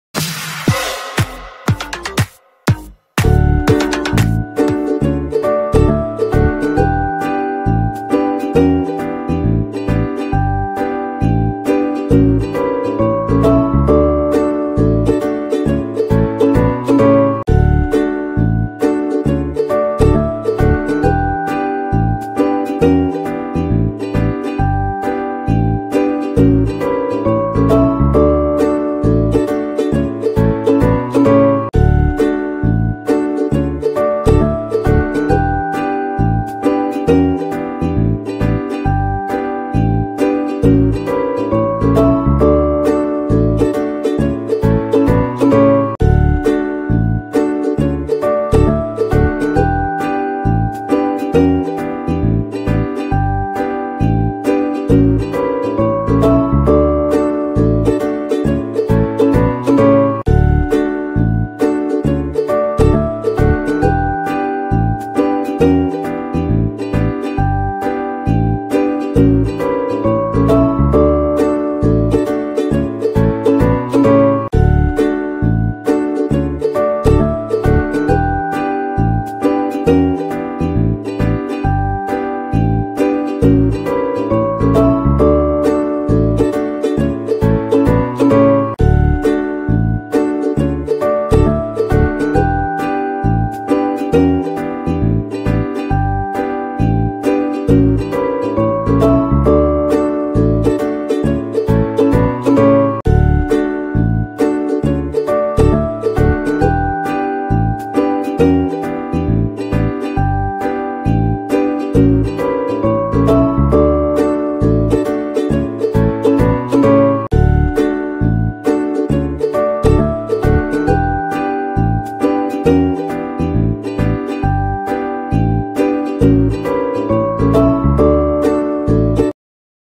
HOW TO MAKE CORN SYRUP /Glucose Syrup ഇനി വീട്ടിൽ തന്നെ ഉണ്ടാക്കാം/Home Made Corn Syrup Recipe
INGREDIENTS :
3/4 CUP SUGAR
3/4 CUP WATER
HALF LEMON